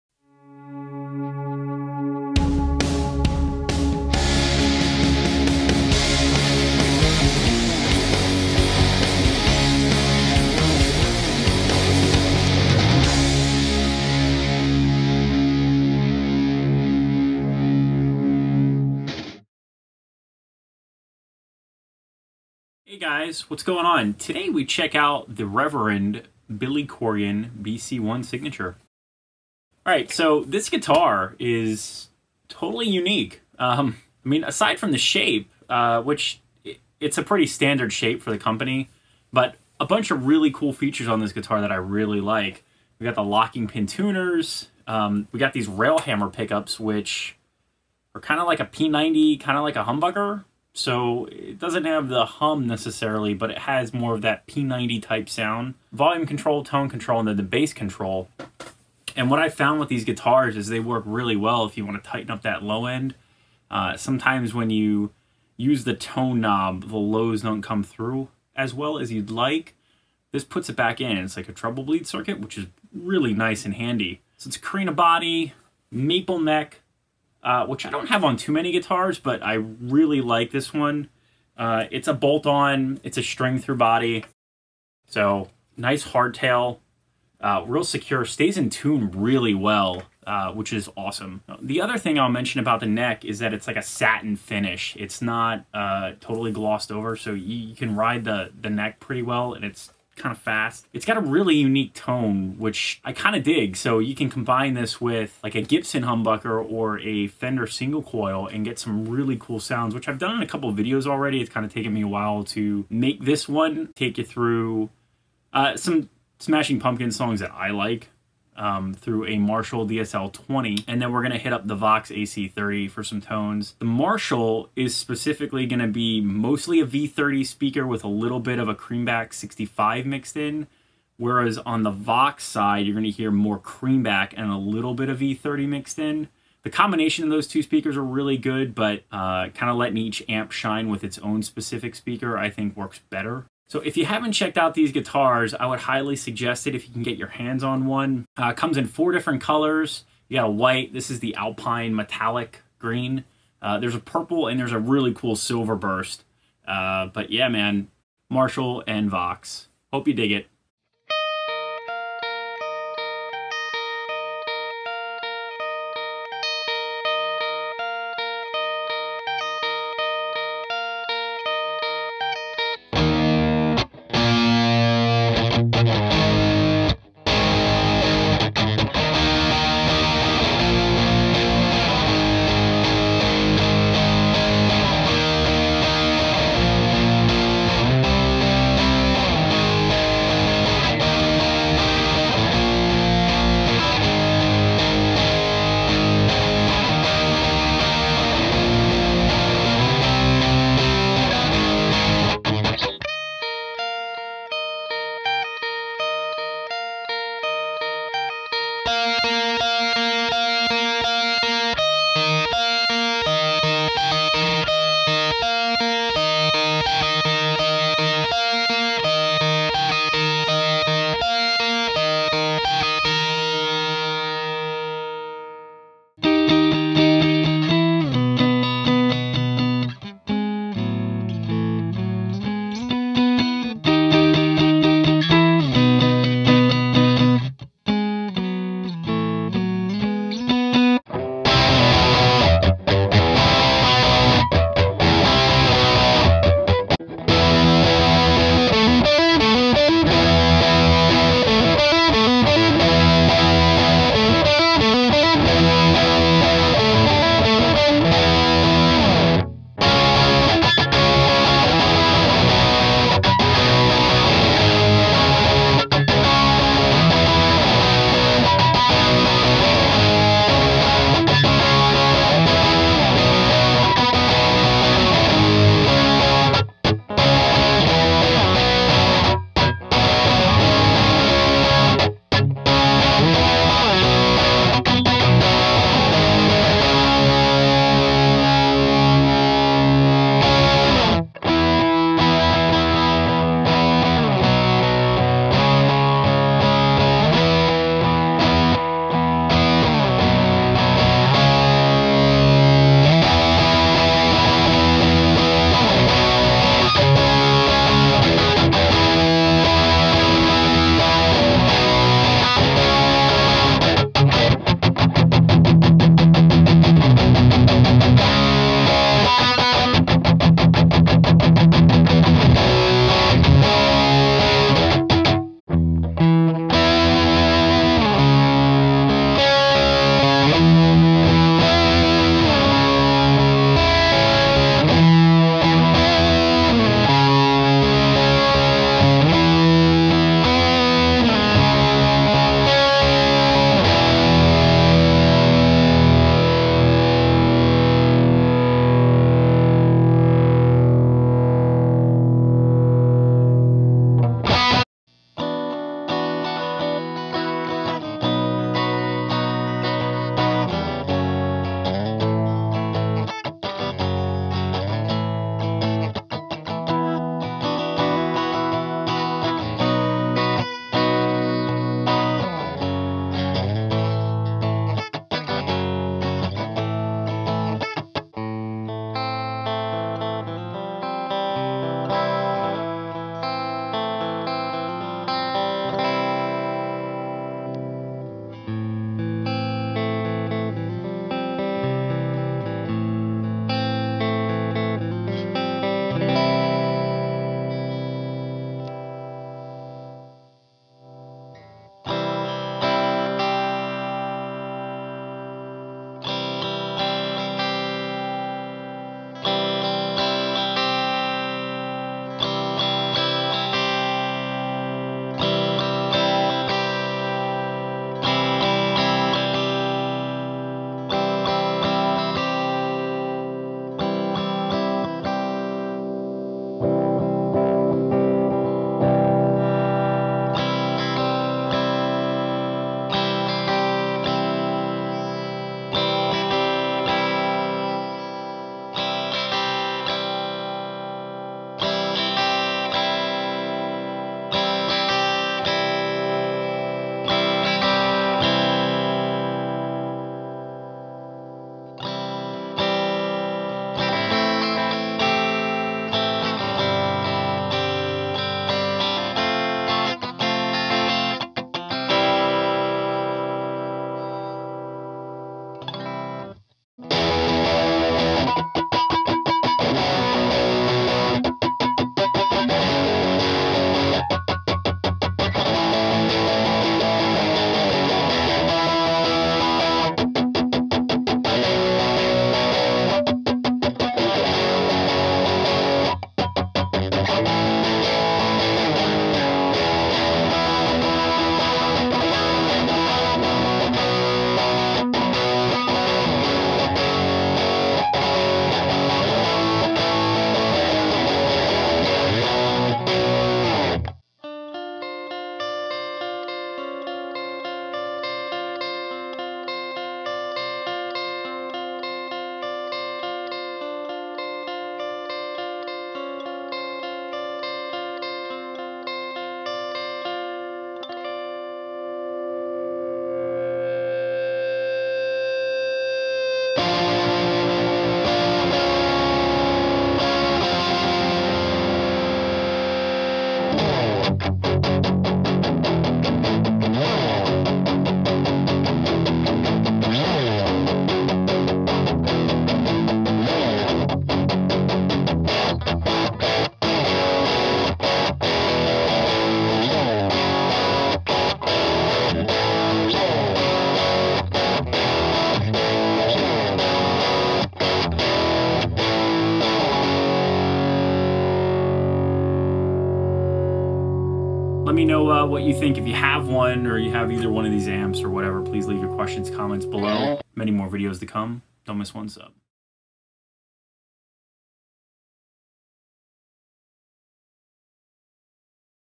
Reverend Billy Corgan Signature Unbox, Review & Demo
Reverend Billy Corgan Signature Unbox, Review & Demo. We talk about this guitar today as well as demo it through two amps - Marshall DSL20HR and the Vox AC30 Handwired.

Have a demo request? Have a question? Just ask!